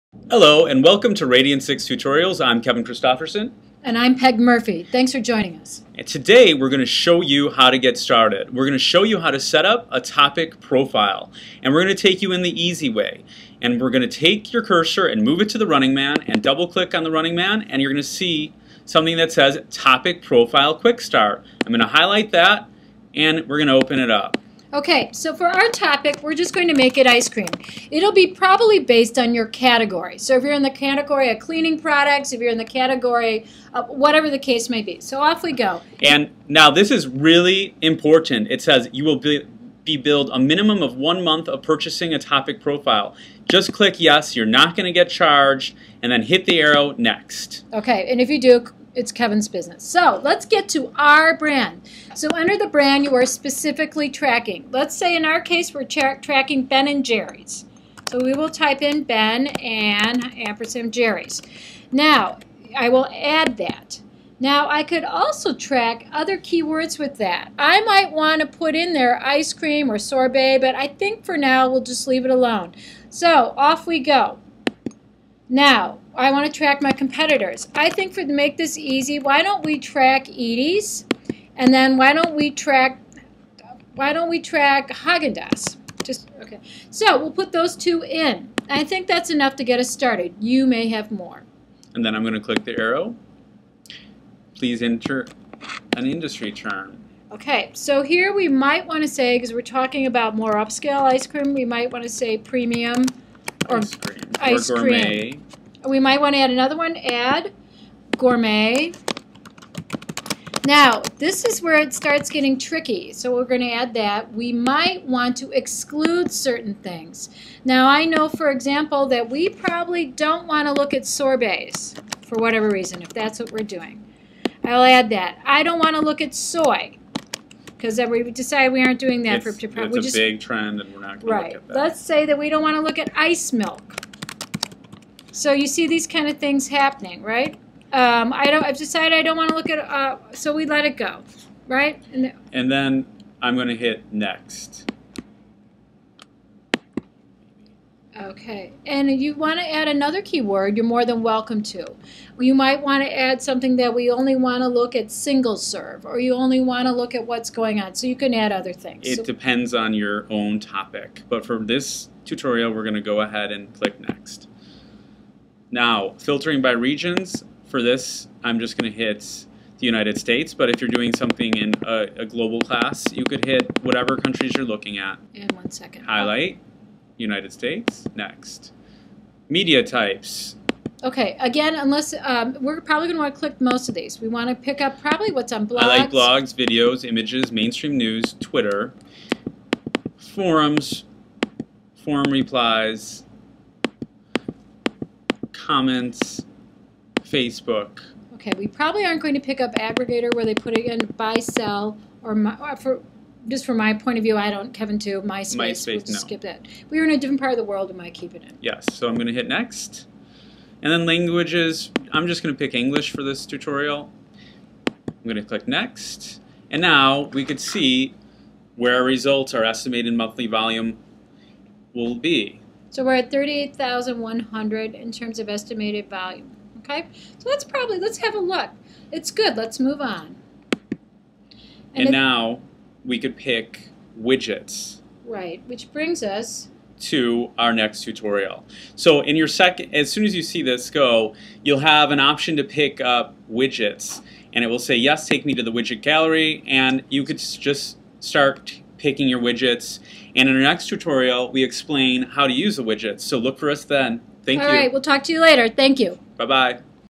FIRST STEP: Setting Up a Topic Profile
In this Radian6 Tutorial, you will learn how to set up a Topic Profile. This is always your first step in the process.